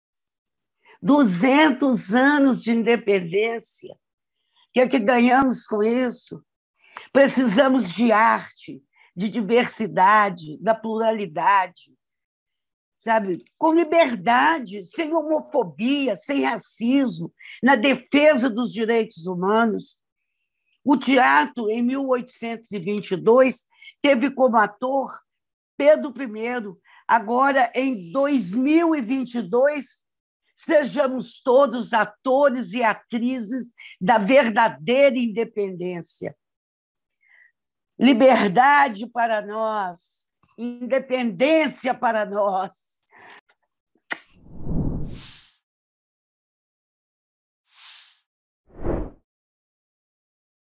Teuda Bara apoia o Portal do Bicentenário
Teuda Bara é uma atriz mineira, de Belo Horizonte. Ela também apoia o Portal do Bicentenário.  Reconhecida pela trajetória teatral como integrante-fundadora do Grupo Galpão, ela tem uma longa lista de personagens atravessada por sua persona inconfundível. Teuda é um ícone do teatro brasileiro.

Para você, qual o papel da arte na independência?

Vem com a gente! Curta, salve, comente, compartilhe!